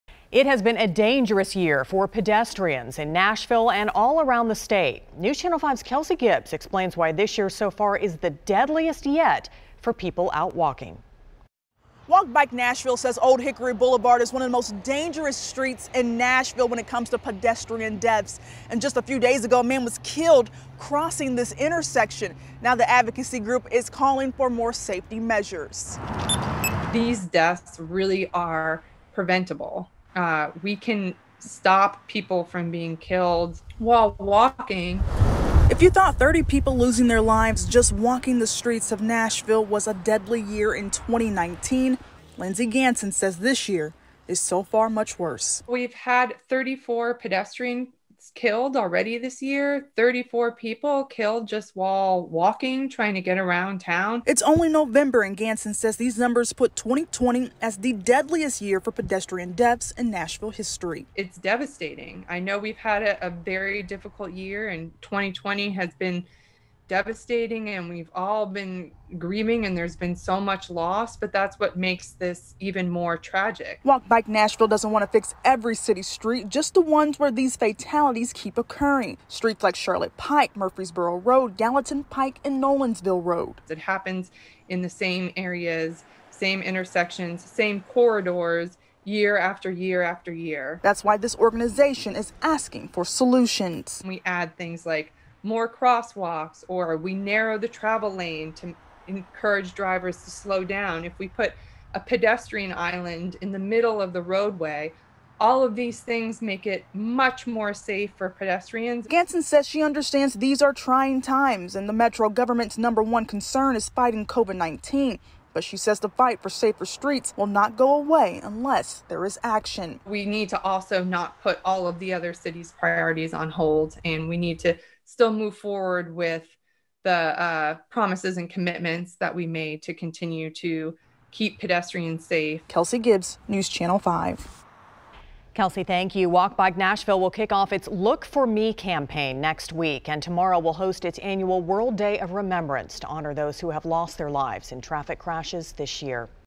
Deadly year for pedestrian crashes in Nashville
The year 2020 is so far the deadliest for pedestrian deaths in Nashville's history